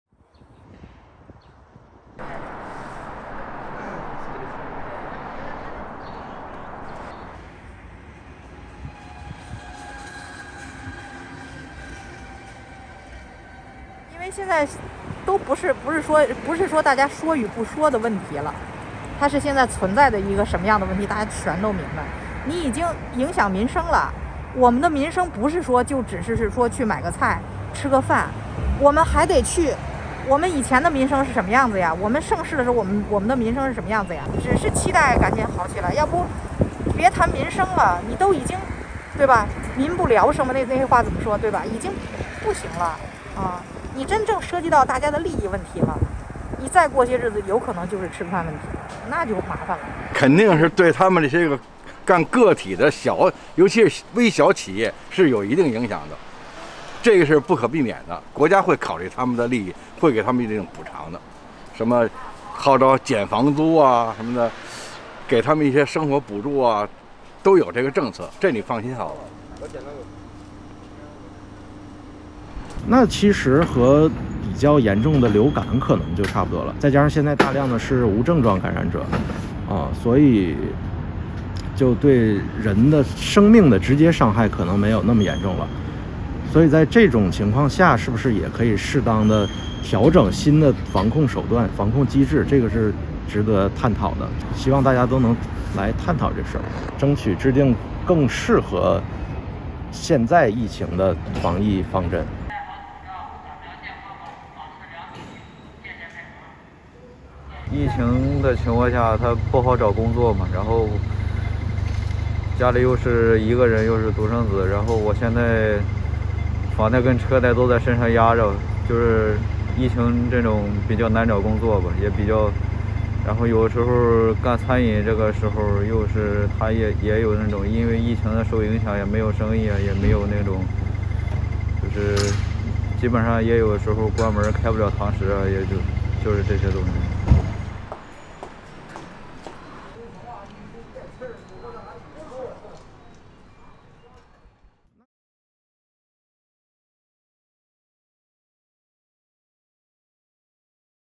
Beijing residents struggle with tightened Covid-19 curbs
The streets in Beijing were near empty on Friday (Nov 25), as China reported another record high of daily Covid-19 infections, prompting cities across the country to enforce measures and curbs to control outbreaks.

Excluding imported infections, China reported 32,695 new local cases for Thursday, a daily record for the second consecutive day.